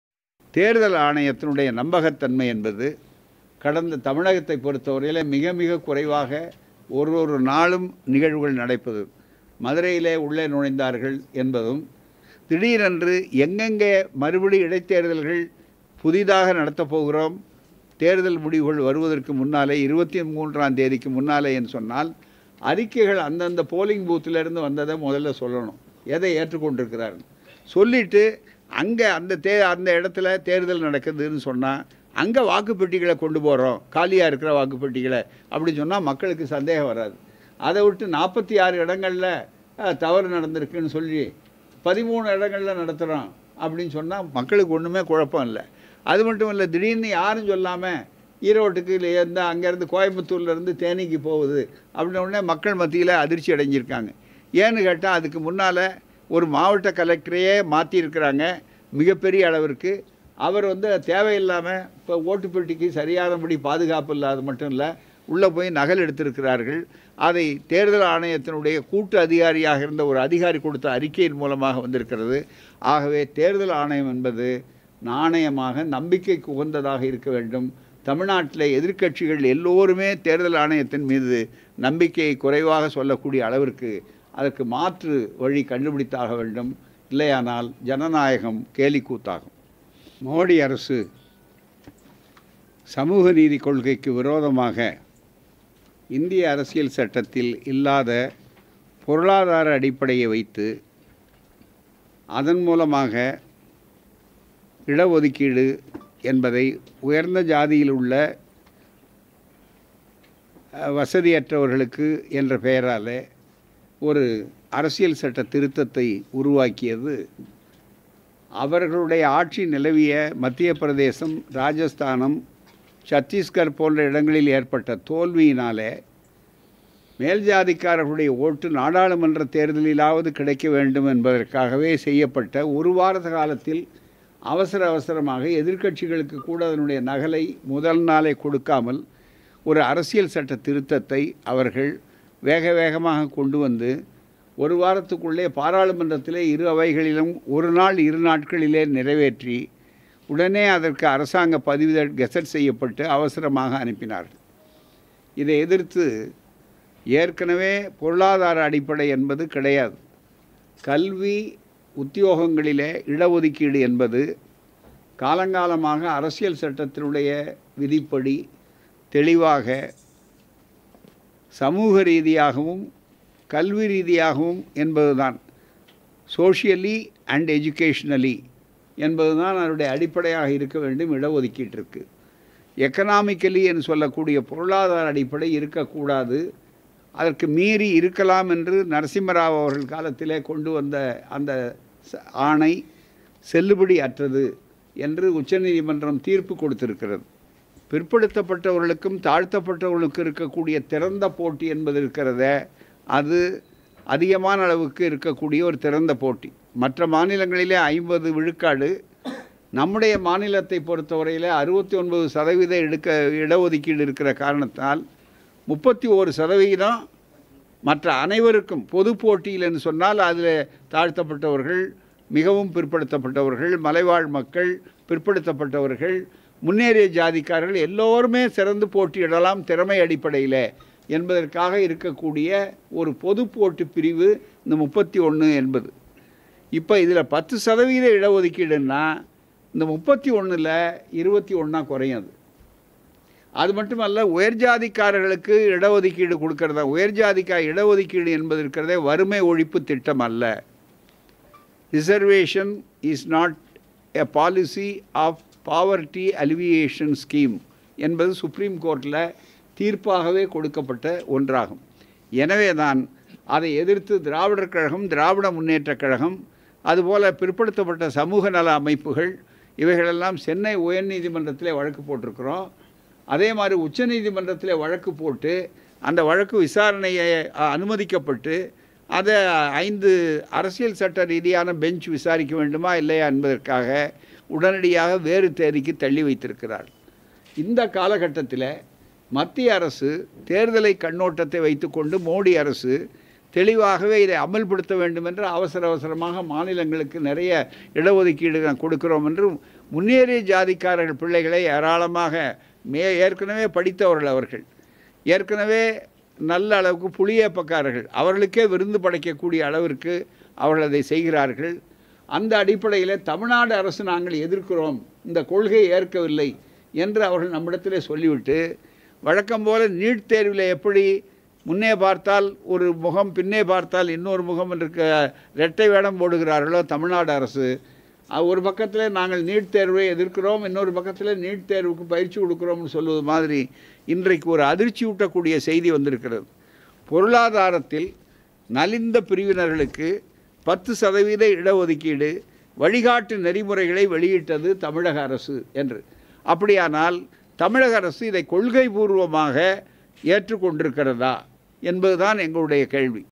|DHARMAPURI| இட ஒதுக்கீடு சட்டம் தமிழக அரசு இரட்டை வேடம் போட்டு வருகிறது கி வீரமணி|
தர்மபுரியில் செய்தியாளர்களிடம் பேசிய திராவிட கழகத் தலைவர் கி வீரமணி #